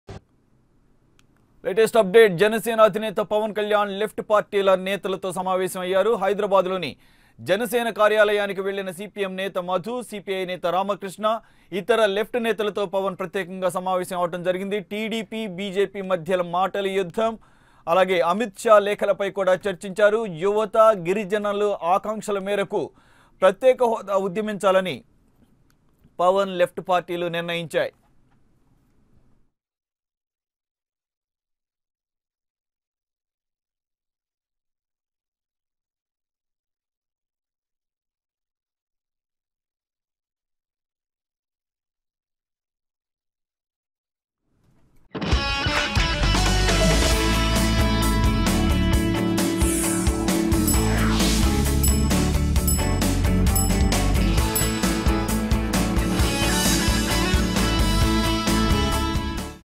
Janasena Chief Pawan Kalyan Meet Left Party Leaders in Hyderabad | AP24x7
Janasena chief Pawan Kalyan meets AP left parties at Janasena party office to discuss on Amit Shah letter to CM Naidu and also regarding the current political scenario. Pawan said the same scene is repeating again & again over TDP and BJP quarrel which clearly shows that AP special status is impossible with CM Naidu.